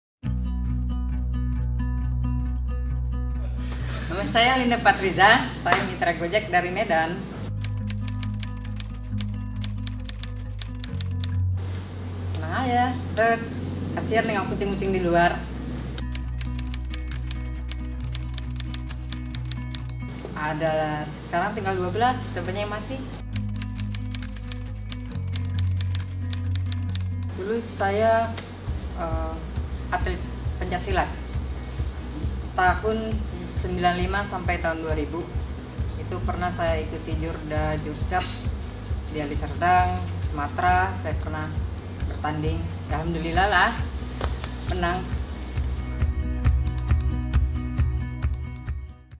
Srikandi Medan - Linda Patriza
Dengan berdirinya GO-JEK di Indonesia, Ibu Linda yang sudah 11 tahun bekerja sebagai Tenaga Kerja Wanita, memutuskan untuk pulang ke tanah air dan bergabung menjadi driver GO-JEK di Medan.